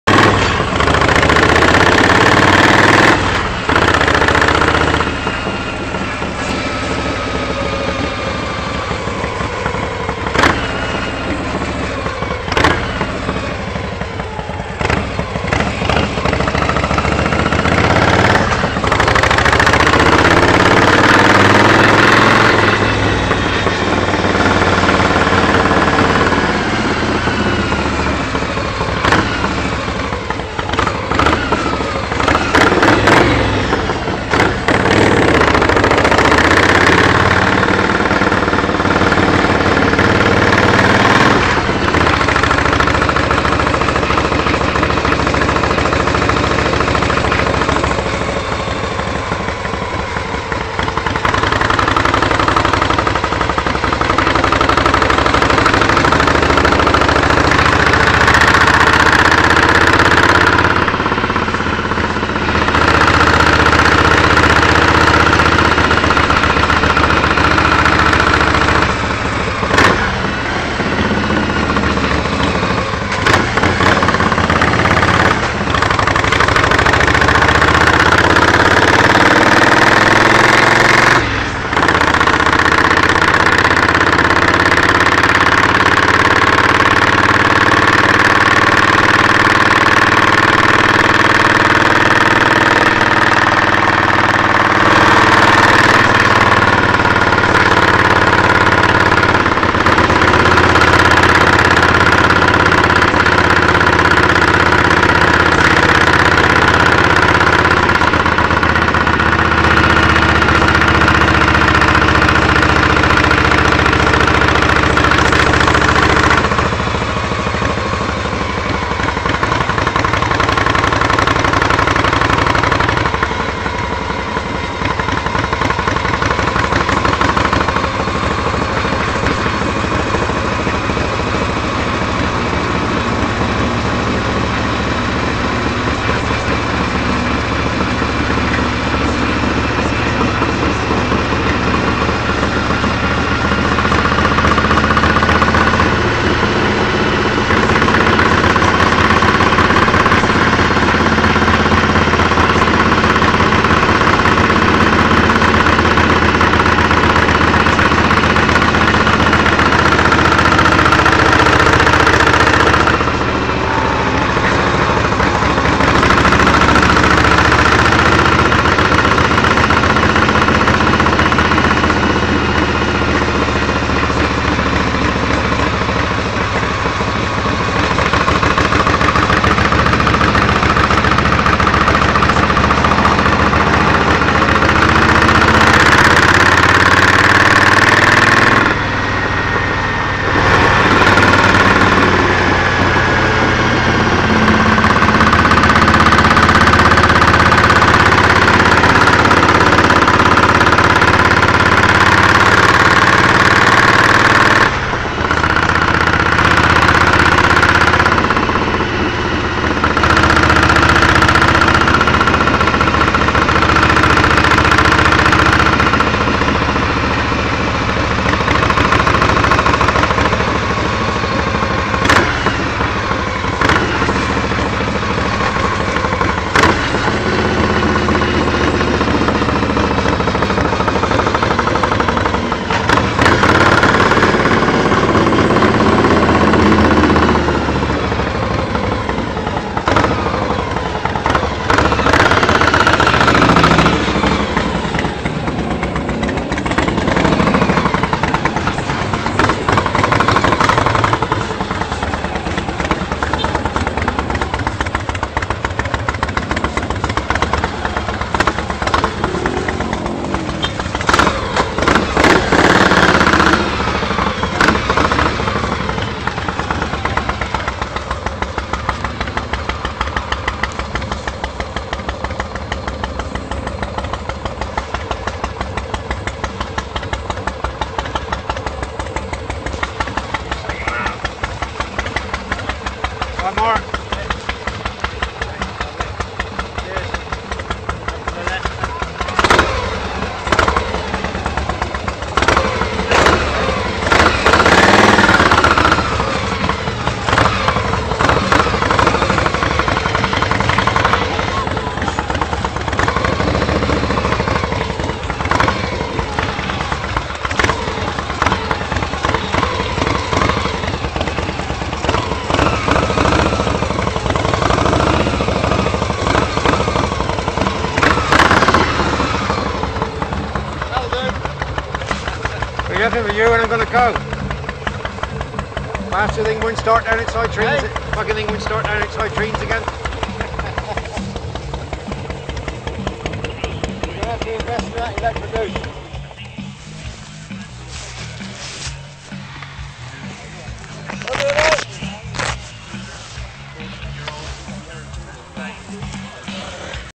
pan sandford
Trip into Sandford bike show 2015 on a 55 year old Harley panhead